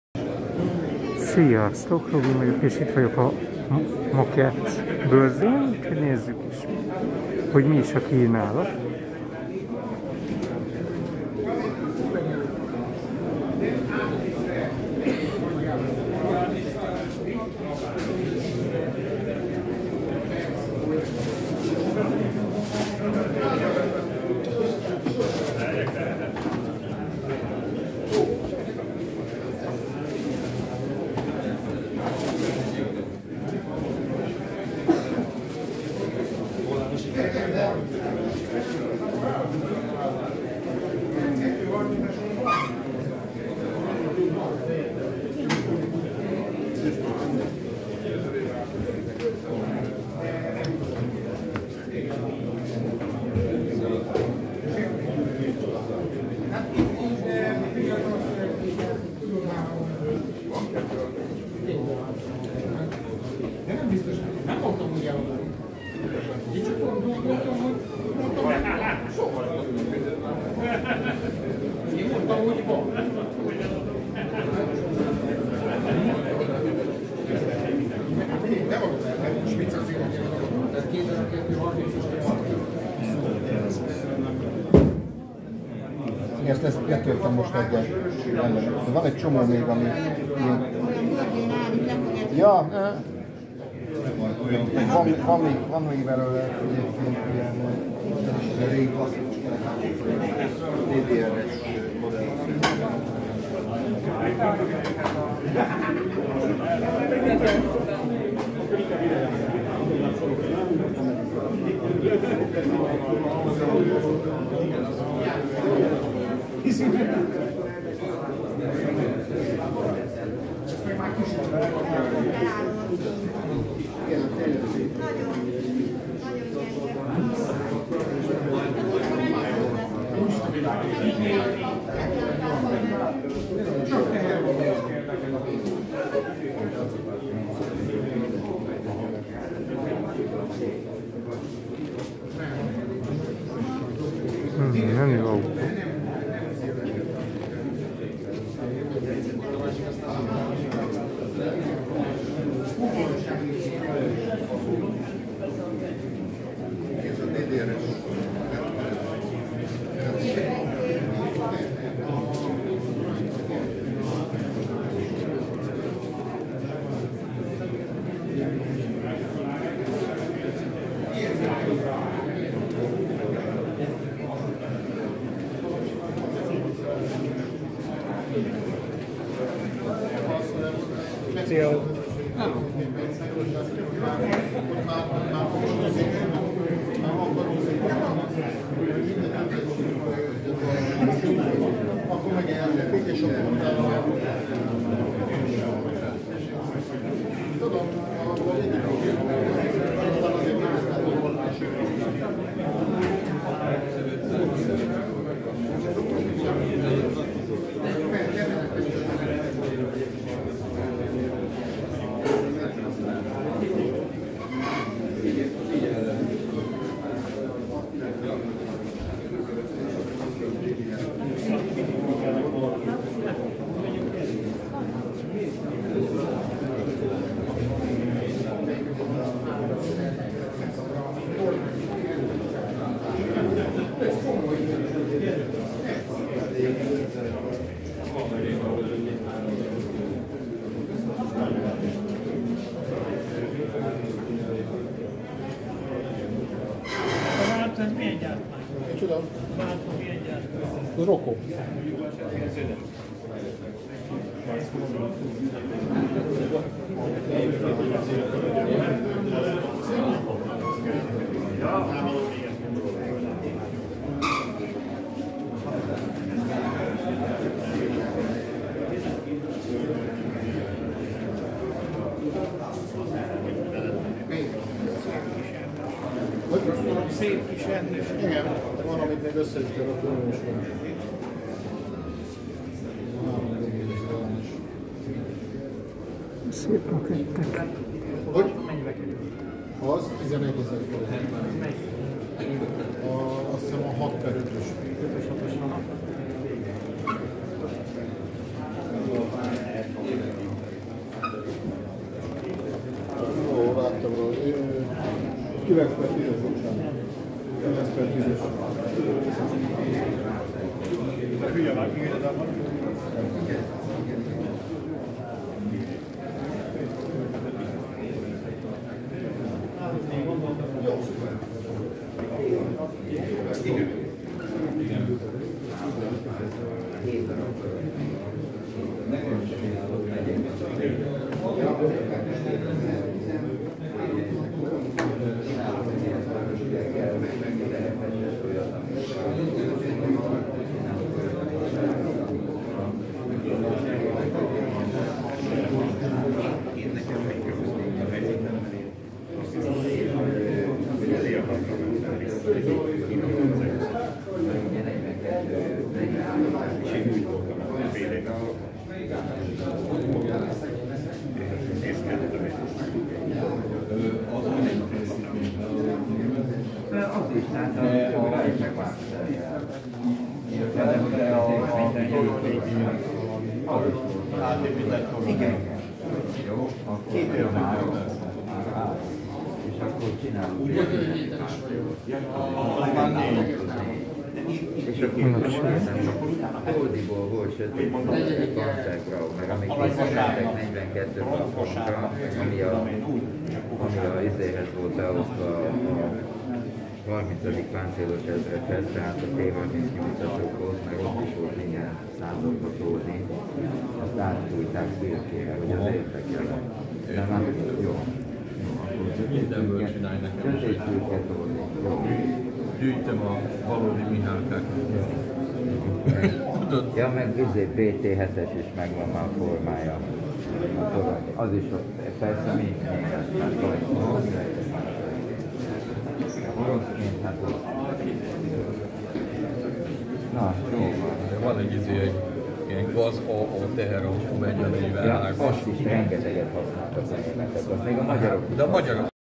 Modellek, makettek, játékok börzéje 2022 (4. Rész)
Az egész művelődési házban börzét tartunk és nem csak vasútmodellezésről fog szólni, hanem minden, ami beletartozhat a játékok kategóriájába. Hajó, autó, repülő, vasút modellek, makettek, legók lesznek!